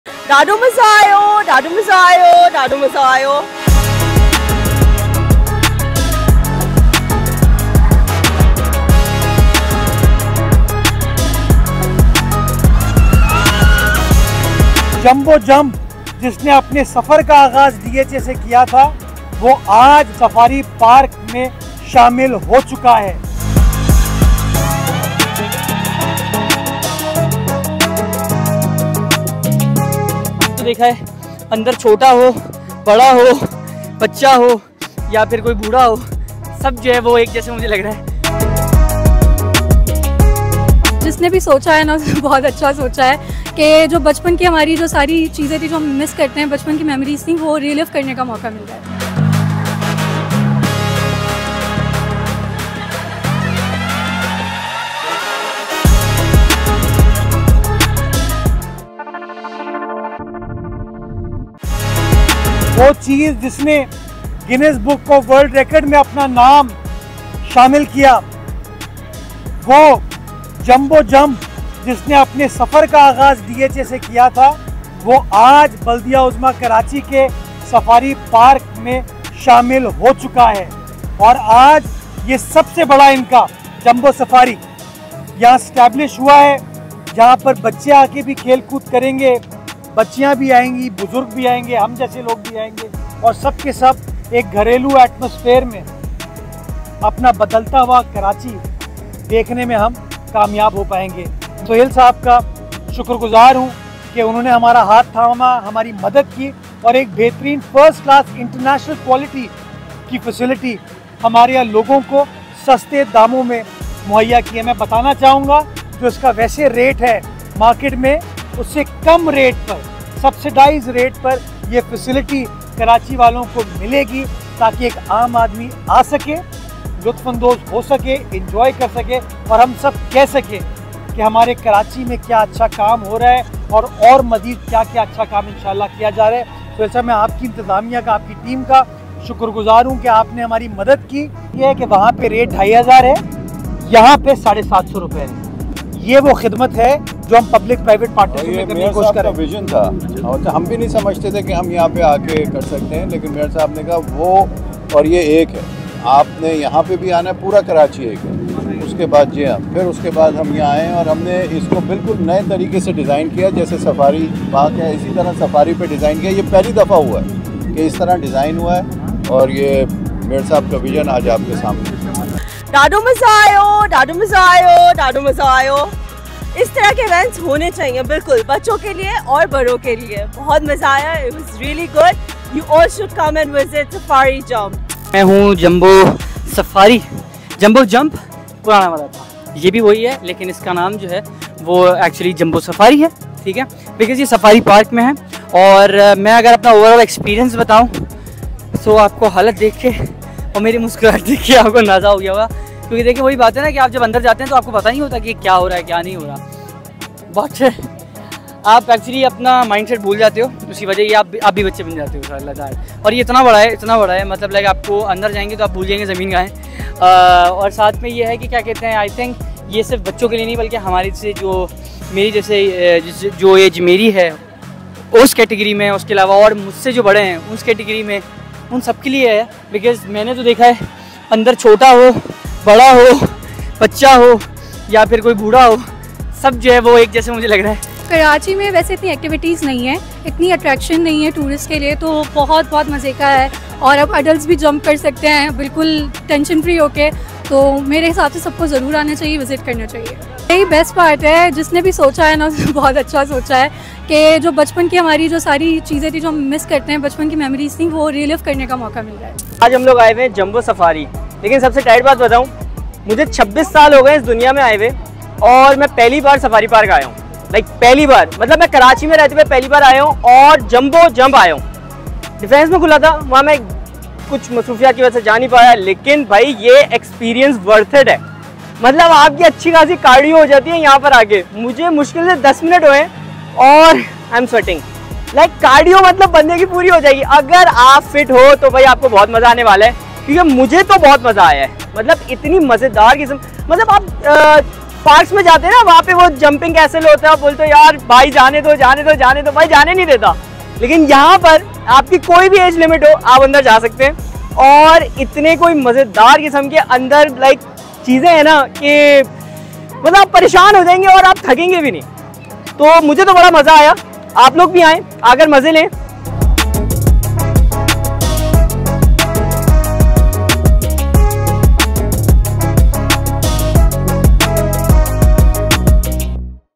Exciting news for Karachiites, Jumbo Jump is now in Safari Park in most economical rate | Karachi
Exciting news for Karachiites, Jumbo Jump is now all set to entertain you at Safari Park in most economical rates.